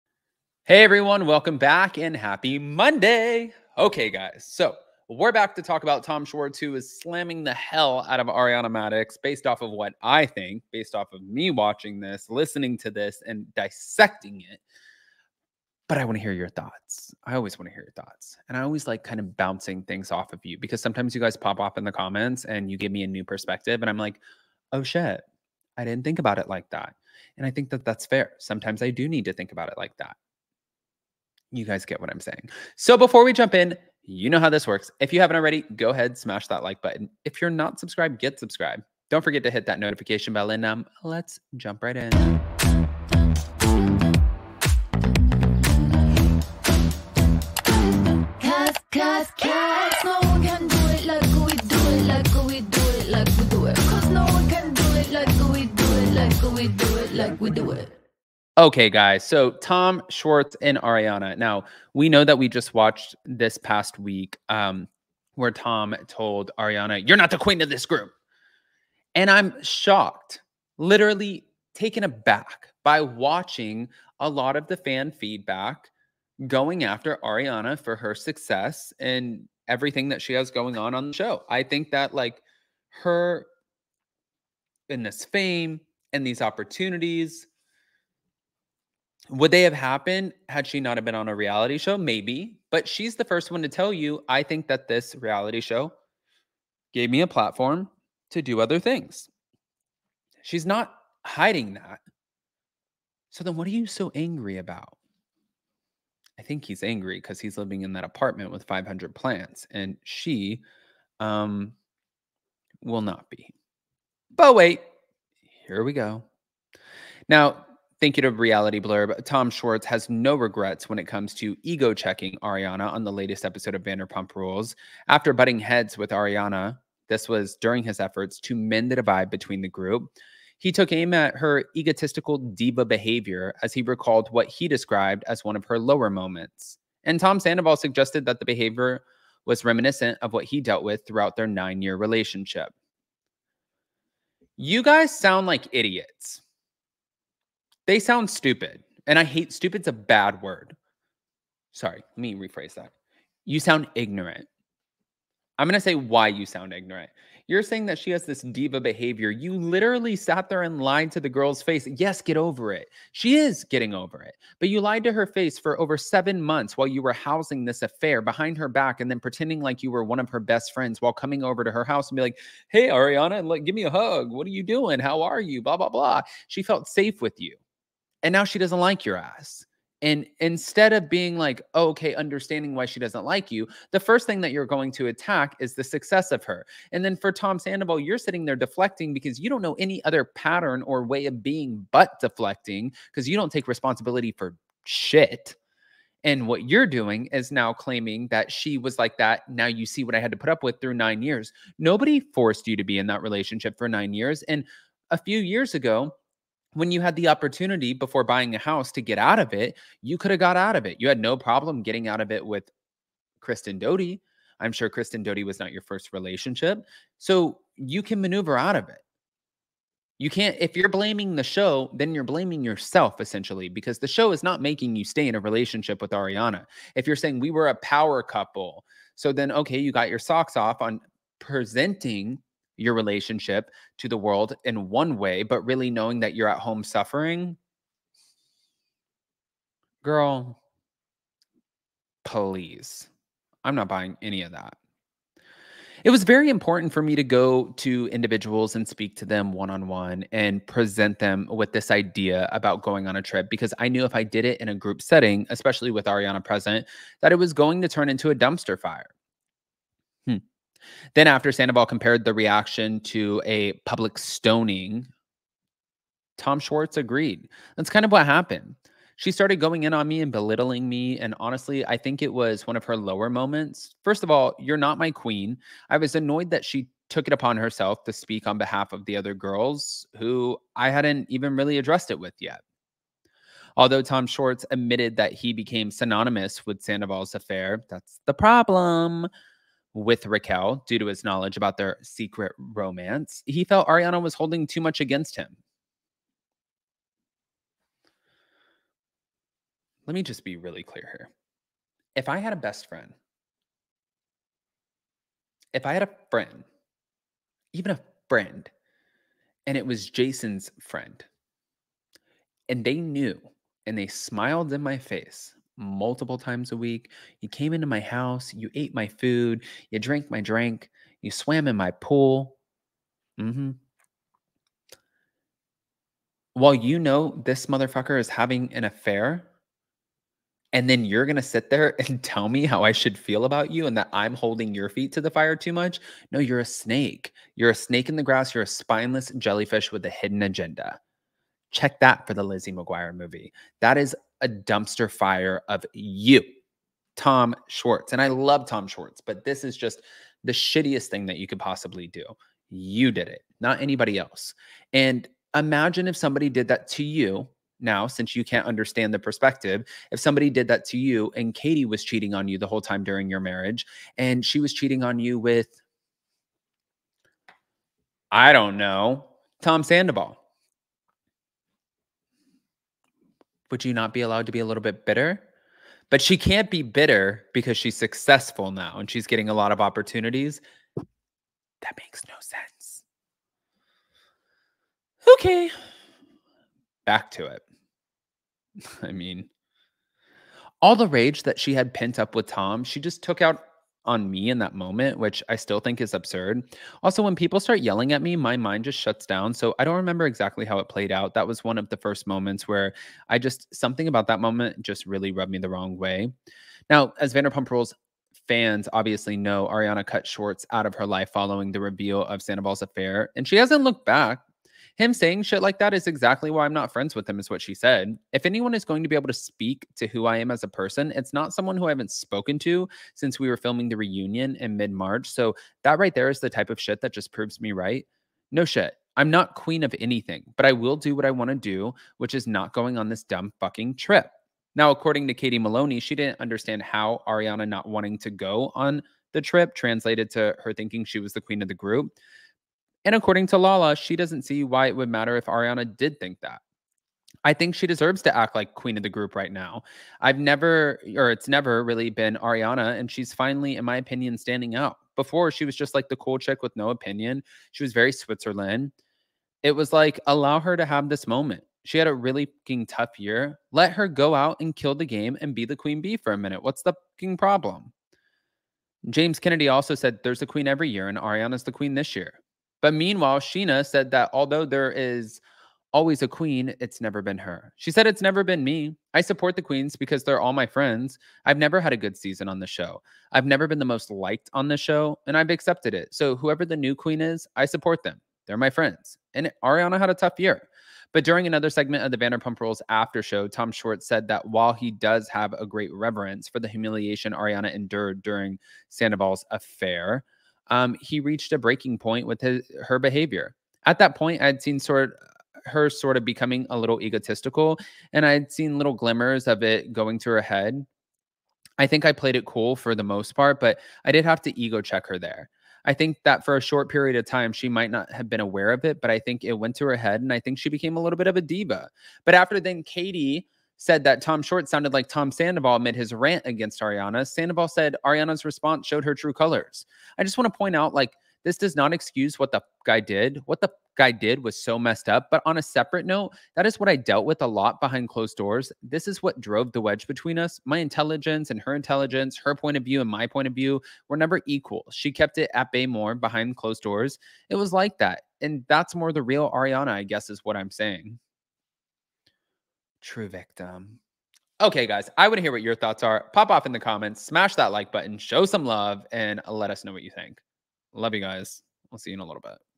Tom Schwartz Slams Ariana Madix In New Interview + Sandoval Weighs In!
🍸 Tom Schwartz and Tom Sandoval have officially put their foots in their own mouths with this one!

- $40 discount off your purchase by using code: Adam40

🎧 Rather listen?

📬 Mail/Packages:
 Up And Adam!
 11231 US HWY 1, Suite 257